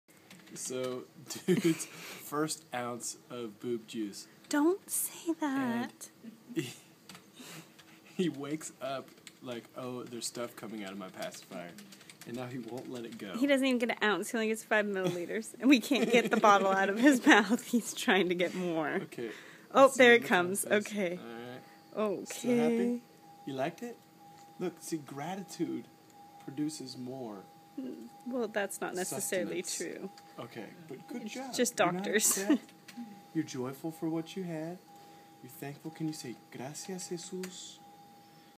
Asa's first nipple nectar
After going the first two weeks of his life outside of the womb, Little dude was pretty impressed to see that some pacifiers have magic that come out them.

He was asleep and we couldn't get him to wake up for his first 5 mL feeding (almost nothing), but then we couldn't pry the bottle from his mouth as his eyes stared at us with stunned delight.

Thank you for your prayer!